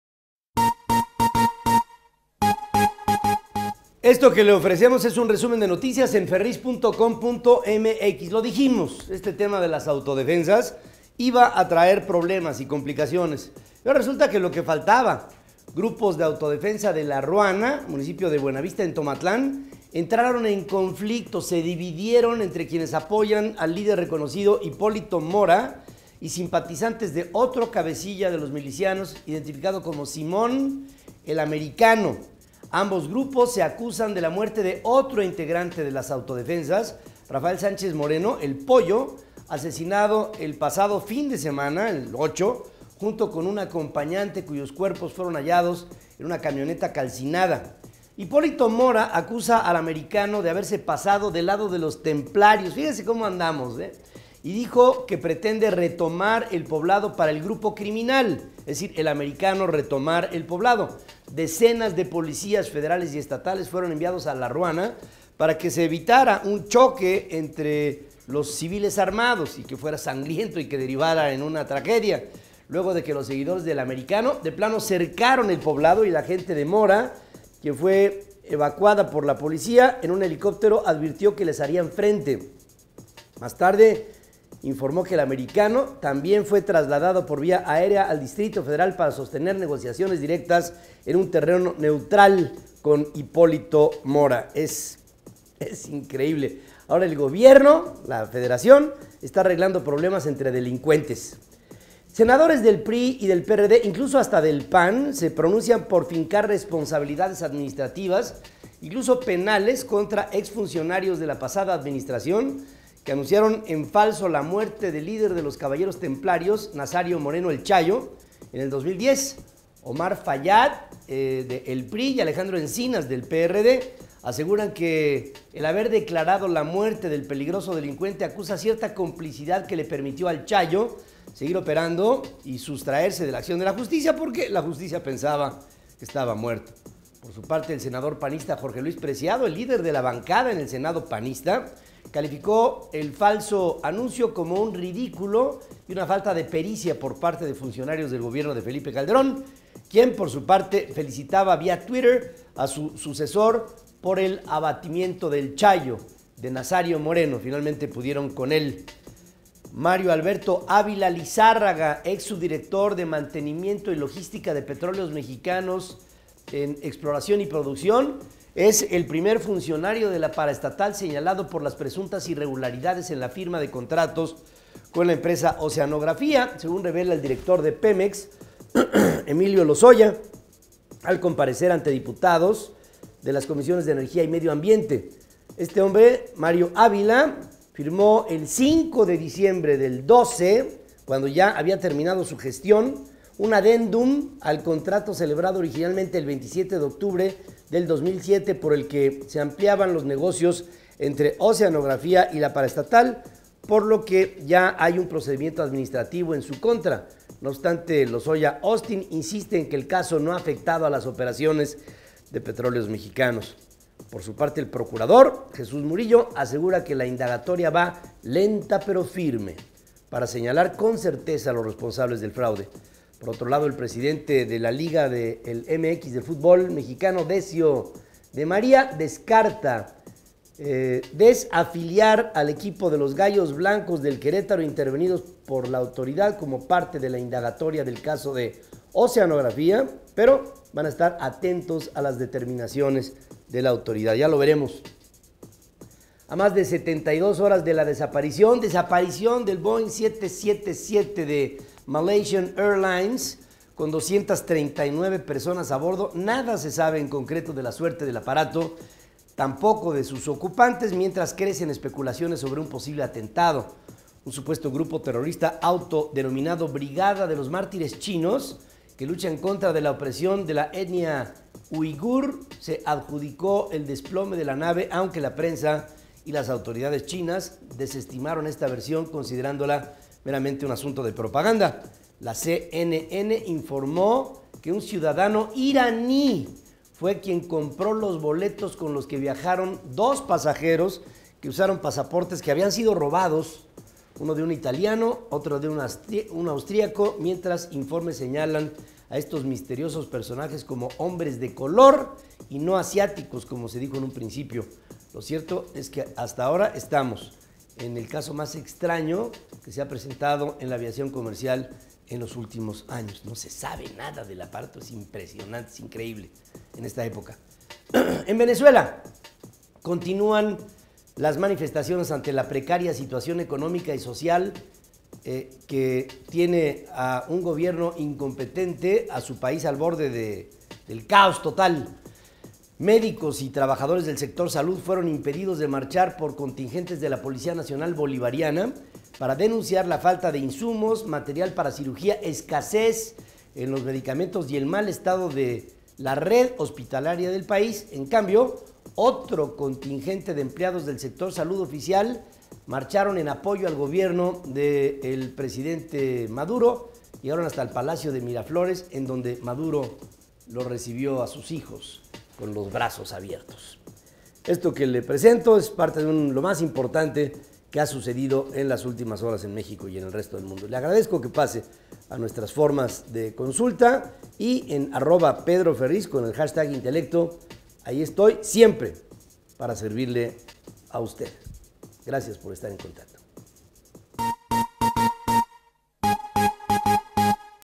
Resumen de noticias
Grupos de autodefensa en La Ruana, Michoacán, entraron en conflicto y se dividieron. Mario Alberto Ávila Lizárraga, exsubdirector de Mantenimiento y Logística de Pemex, es el primer funcionario señalado por presuntas irregularidades con Oceanografía. Esta vez Médicos y trabajadores del sector Salud se manifestaron en Venezuela. Éstas y más noticias aquí.